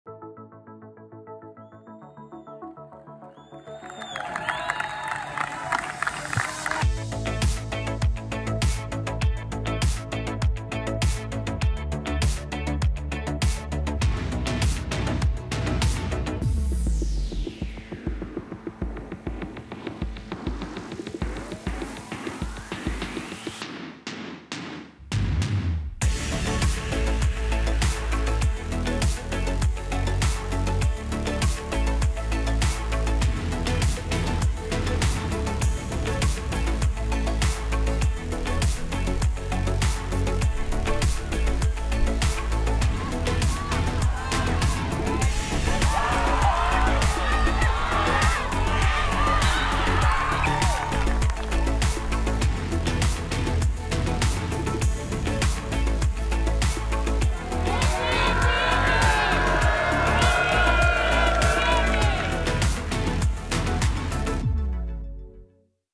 IMART 2022 At Musgrave Park
Get a flavour of the action on another fantastic day at Musgrave Park on Tuesday as the 28 teams competing at the Mixed Ability Rugby World Cup completed their respective pool stages.